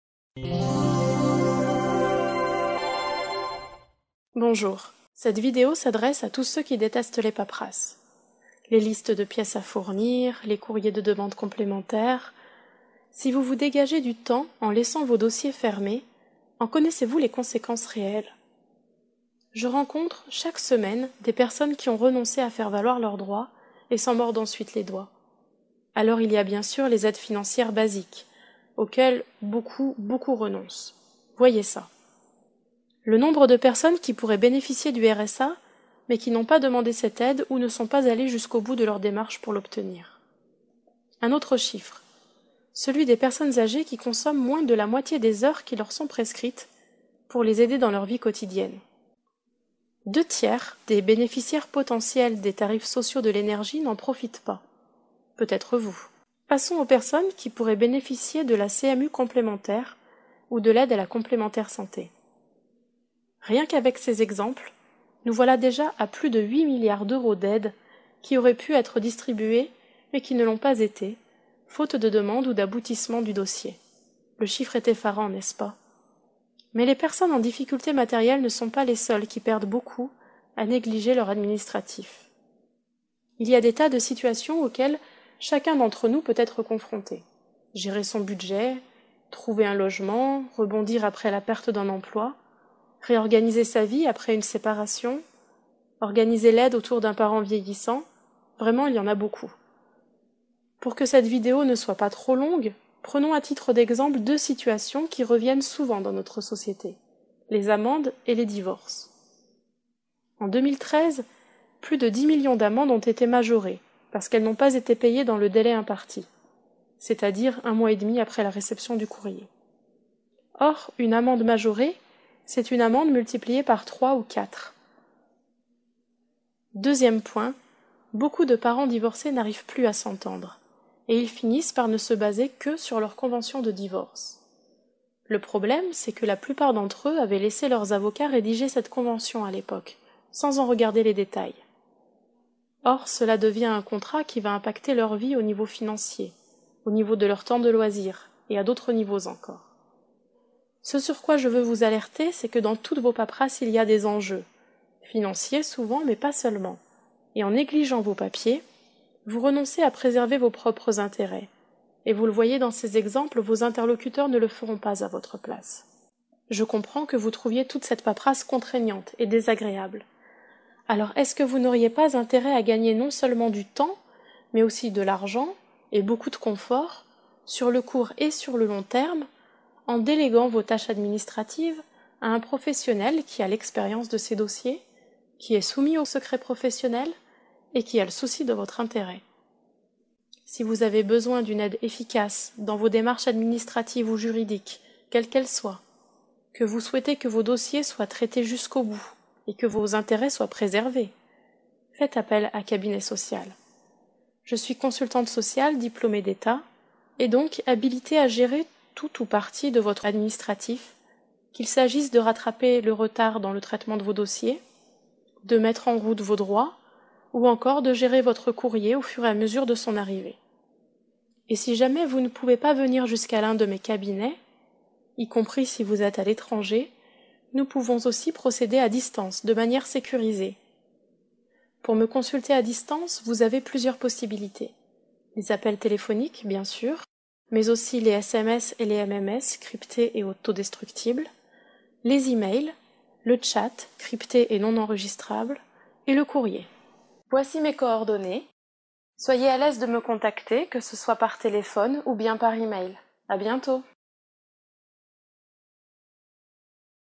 Si vous détestez les paperasses...
Vous en avez marre de tous ces papiers et rêvez d'une aide dans vos démarches administratives ?

Bonjour,

Cette vidéo s’adresse à tous ceux qui détestent les paperasses : les listes de pièces à fournir, les courriers de demandes complémentaires…

Si vous vous dégagez du temps en laissant vos dossiers fermés, en connaissez-vous les conséquences réelles ? Je rencontre chaque semaine des personnes qui ont renoncé à faire valoir leurs droits et s’en mordent ensuite les doigts.

Alors il a y bien sûr les aides financières basiques auxquelles beaucoup, beaucoup renoncent. Voyez ça : le nombre de personnes qui pourraient bénéficier du RSA mais qui n'ont pas demandé cette aide ou ne sont pas allées jusqu'au bout de leur démarche pour l’obtenir.

Un autre chiffre : celui des personnes âgées qui consomment moins de la moitié des heures qui leur sont prescrites pour les aider dans leur vie quotidienne.

Deux tiers des bénéficiaires potentiels des tarifs sociaux de l’énergie n'en profitent pas ; peut-être vous.

Passons aux personnes qui pourraient bénéficier de la CMU complémentaire ou de l'aide à la complémentaire santé. Rien qu'avec ces exemples, nous voilà déjà à plus de 8 milliards d'euros d’aide qui auraient pu être distribués mais qui ne l'ont pas été faute de demande ou d’aboutissement du dossier.

Le chiffre est effarant, n'est-ce pas ?

Mais les personnes en difficulté matérielle ne sont pas les seules qui perdent beaucoup à négliger leur administratif.

Il y a des tas de situations auxquelles chacun d'entre nous peut être confronté : gérer son budget, trouver un logement, rebondir après la perte d'un emploi, réorganiser sa vie après une séparation, organiser l’aide autour d'un parent vieillissant… Vraiment, il y en a beaucoup.

Pour que cette vidéo ne soit pas trop longue, prenons à titre d'exemples deux situations qui reviennent souvent dans notre société : les amendes et les divorces.

En 2013, plus de 10 millions d’amendes ont été majorées, parce qu'elles n'ont pas été payées dans le délai imparti, c'est-à-dire un mois et demi après la réception du courrier. Or, une amende majorée, c'est une amende multipliée par 3 ou 4.

2e point : beaucoup de parents divorcés n'arrivent plus à s’entendre et ils finissent par ne se baser que sur leur convention de divorce.

Le problème, c'est que plupart d'entre eux avaient laissé leurs avocats rédiger cette convention à l’époque, sans en regarder les détails. Or, cela devient un contrat qui va impacter leur vie au niveau financier, au niveau de leur temps de loisirs et à d'autres niveaux encore.

Ce sur quoi je veux vous alerter, c'est que dans toutes vos paperasses, il y a des enjeux, financiers souvent, mais pas seulement. Et en négligeant vos papiers, vous renoncez à préserver vos propres intérêts. Et vous le voyez dans ces exemples, vos interlocuteurs ne le feront pas à votre place.

Je comprends que vous trouviez toute cette paperasse contraignante et désagréable.

Alors, est-ce que vous n'auriez pas intérêt à gagner non seulement du temps, mais aussi de l'argent et beaucoup de confort, sur le court et sur le long terme, en déléguant vos tâches administratives à un professionnel qui a l'expérience de ces dossiers, qui est soumis au secret professionnel et qui a le souci de votre intérêt ?

Si vous avez besoin d'une aide efficace dans vos démarches administratives ou juridiques, quelles qu’elles soient, que vous souhaitez que vos dossiers soient traités jusqu'au bout et que vos intérêts soient préservés, faites appel à Cabinet Social.

Je suis consultante sociale diplômée d’Etat et donc habilitée à gérer tout ou partie de votre administratif, qu’il s’agisse de rattraper le retard dans le traitement de vos dossiers, de mettre en route vos droits ou encore de gérer votre courrier au fur et à mesure de son arrivée.

Et si jamais vous ne pouvez pas venir jusqu'à l'un de mes cabinets, y compris si vous êtes à l’étranger, nous pouvons aussi procéder à distance, de manière sécurisée.

Pour me consulter à distance, vous avez plusieurs possibilités : les appels téléphoniques, bien sûr, mais aussi les SMS et les MMS cryptés et auto-destructibles, les e-mails, le tchat crypté et non-enregistrable et le courrier.

Soyez à l’aise de me contacter, que ce soit par téléphone ou bien par e-mail. A bientôt !

Stéphanie LADEL, pour Cabinet Social